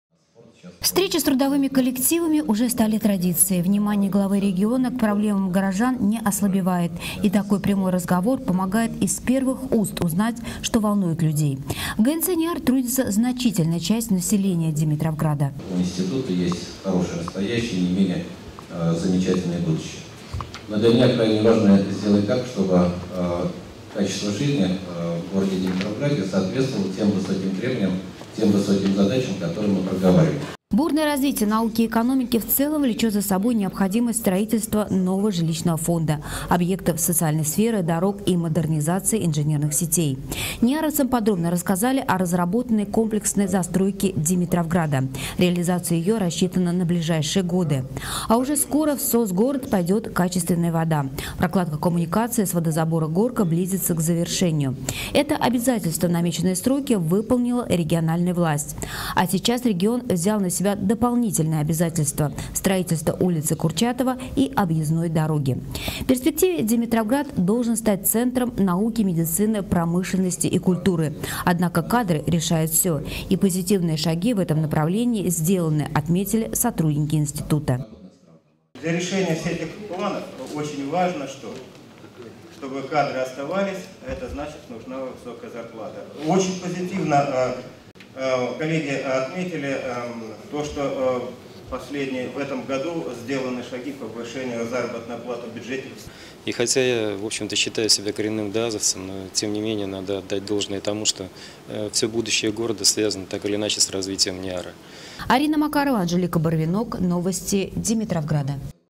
Откровенный разговор
В среду, в рамках рабочего визита в Димитровград, губернатор посетил ГНЦ НИИАР. В конференц-зале состоялась встреча с сотрудниками института, сообщает